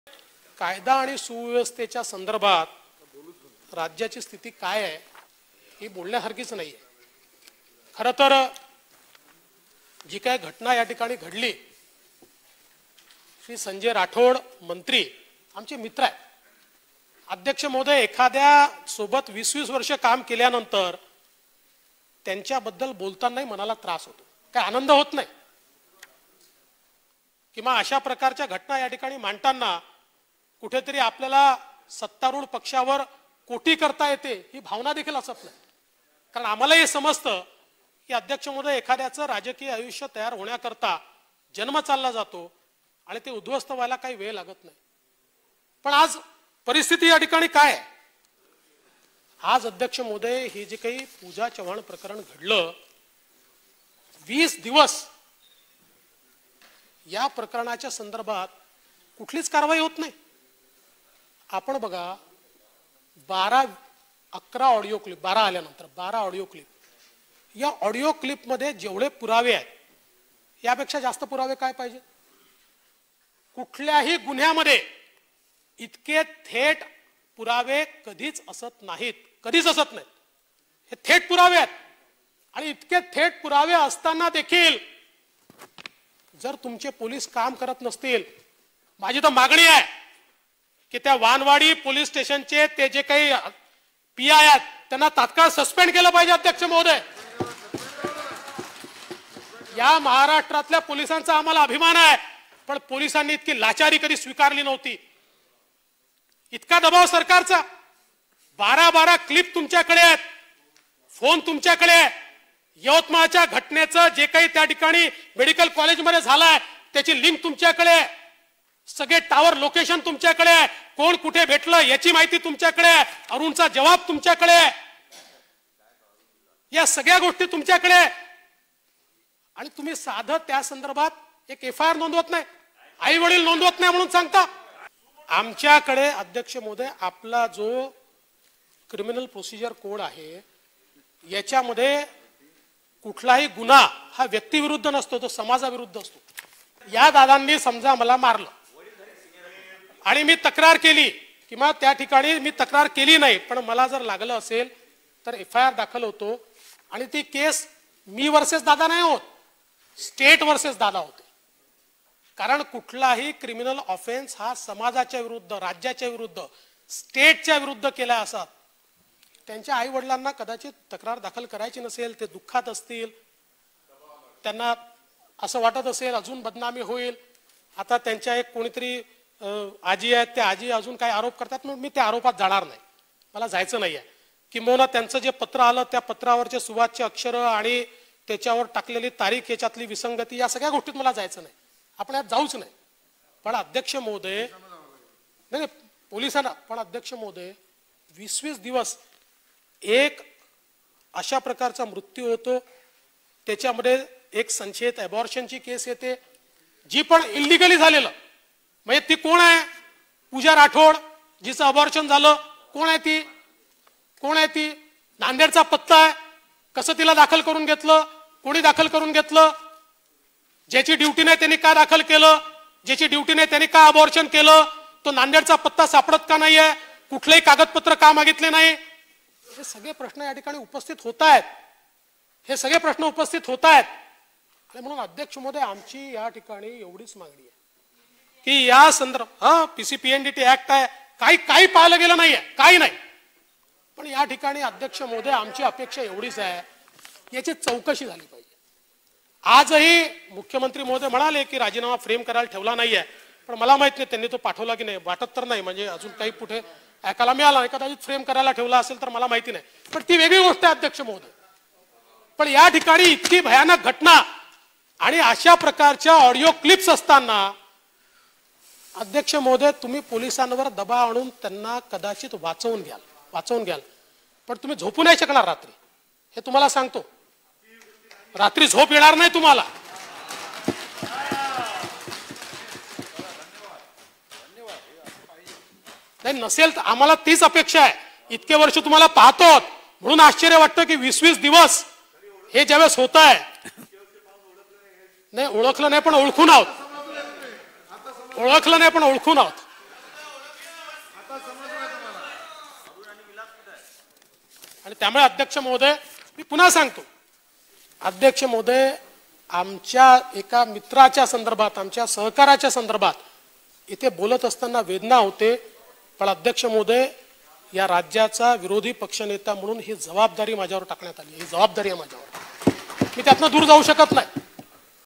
पूजा चव्हाण प्रकरणात सर्व पुरावे असून पोलिस गुन्हा दाखल करत नसतील, तर निलंबित करा त्या अधिकाऱ्याला!
पूजा चव्हाण प्रकरणात सर्व पुरावे असून पोलिस गुन्हा दाखल करत नसतील, तर निलंबित करा त्या अधिकाऱ्याला!(राज्यपाल अभिभाषणातील चर्चा । महाराष्ट्र विधानसभा । दि. 2 मार्च 2021)

कायदा आणि सुव्यवस्थेच्या संदर्भात राज्याची स्थिती काय हे बोलण्यासारखे नाही. मंत्री संजय राठोड आमचे मित्र आहेत. एखाद्या सोबत 20 वर्षे काम केल्यानंतर त्यांच्याबद्दल बोलतानाही मनाला त्रास होतो. आनंद होत नाही. अशा प्रकारच्या घटना मांडताना कुठेतरी सत्तारूढ पक्षावर कोटी करता येते ही भावना देखील मनामध्ये नाही. आम्हालाही समजते एखाद्याचे राजकीय आयुष्य तयार होण्याकरिता जन्म जातो आणि ते उध्वस्त व्हायला वेळ लागत नाही. पण आज परिस्थिती काय आहे? जे पूजा चव्हाण प्रकरण घडले, 20 दिवस या प्रकरणात कुठलीच कारवाई होत नाही. 12 ऑडिओ क्लिपमध्ये जेवढे पुरावे आहे त्यापेक्षा अजून काय पुरावे पाहिजेत? कुठल्याही गुन्ह्यामध्ये इतके थेट पुरावे कधीच असत नाहीत. हे थेट पुरावे आहेत. इतके थेट पुरावे असताना देखील पोलीस काम करत नसतील तर माझी मागणी आहे की, त्या वानवाडी पोलीस स्टेशनचे जे काही पीआय आहेत त्यांना तात्काळ सस्पेंड केले पाहिजे. महाराष्ट्रातील पोलिसांचा आम्हाला अभिमान आहे. पण पोलिसांनी इतकी लाचारी कधी स्वीकारली नव्हती.  इतका दबाव सरकारचा?  12-12 क्लिप तुमच्याकडे आहेत, फोन तुमच्याकडे, यवतमाळ मेडिकल कॉलेजमध्ये जे झाले त्याची लिंक तुमच्याकडे, सगळे टॉवर  लोकेशन तुमच्याकडे, कोण कुठे भेटले याची माहिती, अरुणचा जवाब तुमच्याकडे असूनही साधी एफआयआर नोंदवित नाही. आईवडील नोंदवित नाही सांगता, कुणाच्या तक्रारीची आवश्यकता नाही. आपला जो क्रिमिनल प्रोसिजर कोड आहे त्यामध्ये गुन्हा व्यक्ती विरुद्ध नसतो तो समाजाविरुद्ध असतो. समजा उद्या मला जर दादांनी मारले, मी तक्रार केली नाही, पण मला लागले असेल तर ती केस 'मी विरुद्ध दादा' नाही होत ती 'स्टेट विरुद्ध दादा' अशी होते. कारण कुठलाही क्रिमिनल ऑफेन्स हा समाजाविरुद्ध, राज्याच्याविरुद्ध केला आहे असा होतो. त्यांच्या आई वडिलांना तक्रार करायची नसेल, ते दुःखात असतील. त्यांना वाटत असेल अजून बदनामी होईल. त्यांच्या कुणीतरी आजी आहेत त्या आरोप करतात. मी त्याच्यात जाणार नाही किंबहुना त्यांचे पत्रावरचे सुवाच्च अक्षर, त्याच्यात टाकलेली तारीख यातील विसंगती आपण याविषयी बोलूच नये.
पण 20-20 दिवस एक मृत्यू होतो. अवैध पद्धतीने झालेल्या संशयित गर्भपाताची केस येते. ती कोण आहे पूजा राठोड जिचा गर्भपात झाला? कोण आहे ती ? नांदेडचा पत्ता आहे. कसे तिला दाखल करुन घेतले ? कुणी दाखल केले ? ज्याची ड्युटी नाही त्याने का दाखल केले? ज्याची ड्युटी नाही त्याने का गर्भपात केला? तो नांदेडचा पत्ता सापडत का नाही ? कुठलेही कागदपत्र का मागितले नाही? सगळे प्रश्न उपस्थित होत आहेत. पीसीपीएनडीटी कायदा आहे. काही पाहिले गेले नाही. आमची मागणी आहे याची चौकशी झाली पाहिजे. मुख्यमंत्री म्हणाले राजीनामा फ्रेम करायला ठेवला नाही. मला माहित नाही त्यांनी तो पाठविला की नाही, वाटत नाही अजून कुठे ऐकायला नाही आले. कदाचित फ्रेम करायला ठेवला असेल तर मला माहित नाही. इतकी भयानक घटना अशा प्रकारच्या ऑडिओ क्लिपस् असताना. तुम्ही पोलिसांवर दबाव आणून त्यांना कदाचित वाचवून घ्याल पण तुम्ही झोपू शकणार नाही, रात्री झोप येणार नाही तुम्हाला. इतके वर्ष तुम्हाला पाहतोय. ओळखले नाही पण ओळखून आहोत. त्यामुळे अध्यक्ष महोदय, मी पुन्हा सांगतो आमच्या मित्रासंदर्भात, सहकाऱ्यासंदर्भात बोलत असताना वेदना होते. पण राज्याचा विरोधी पक्ष नेता म्हणून ही जबाबदारी माझ्यावर टाकण्यात आली आहे. ही जबाबदारी आहे माझ्यावर मी त्यातून दूर जाऊ शकत नाही.